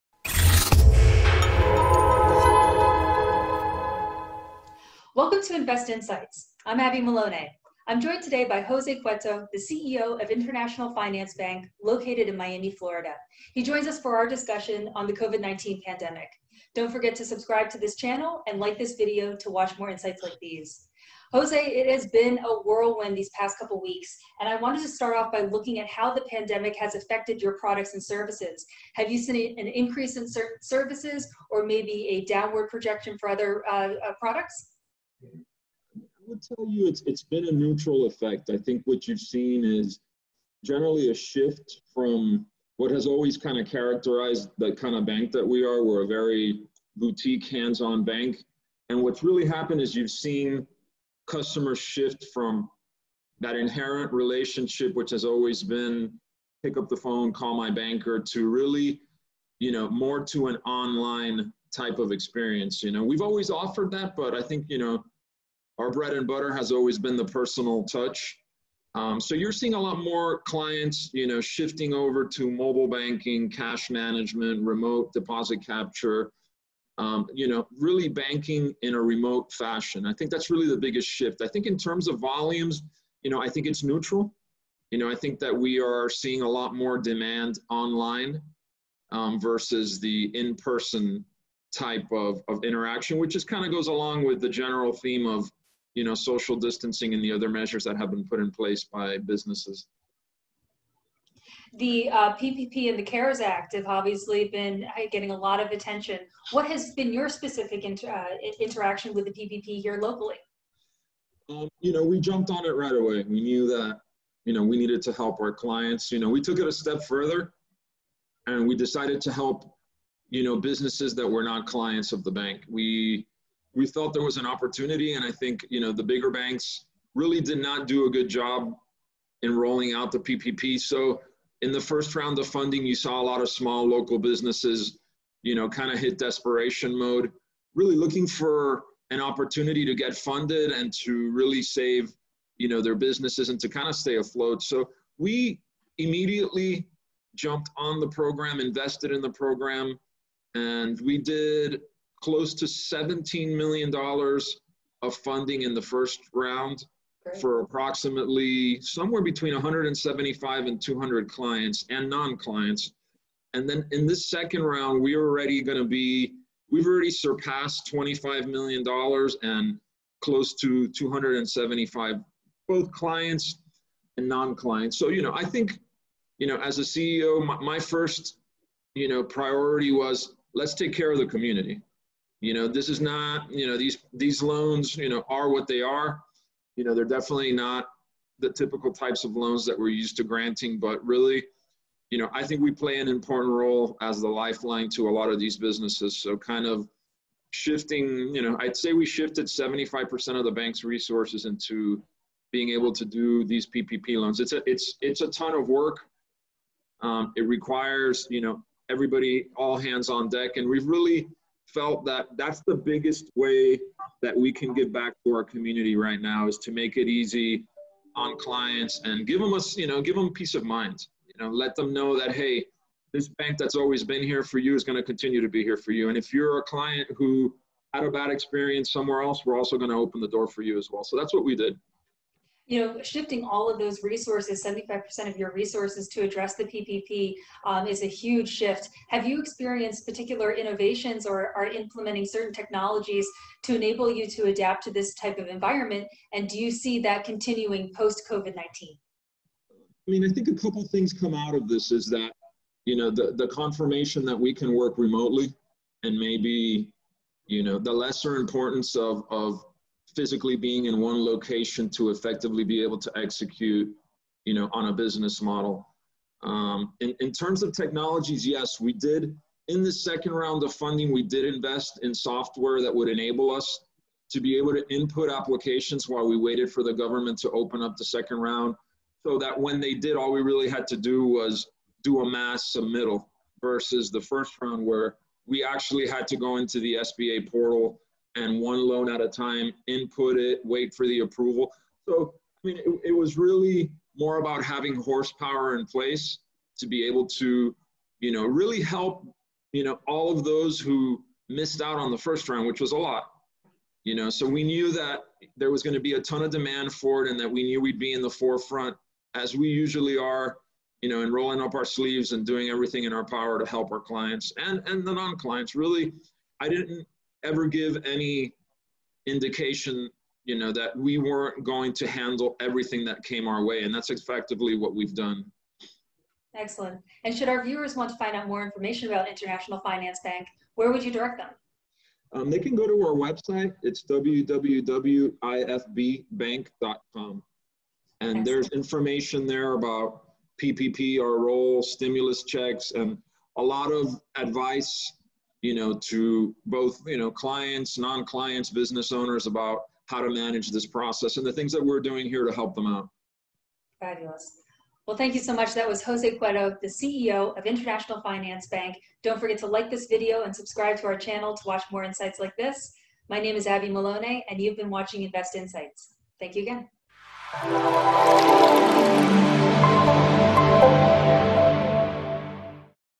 Forward thinking gave this bank an advantage in processing PPP loan applications
Since the passage of the CARES Act, banks around the nation have been inundated with PPP loan applications. Jose Cueto, the CEO of International Finance Bank who was recently appointed to the City of Miami's COVID-19 recovery board, spoke to Abby Melone about his bank's forward thinking and planning as it relates to the PPP loan applications and how his bank is helping both clients and non clients in submitting and processing these loan applications.

For a limited amount of time, register to read all Capital Analytics reports for free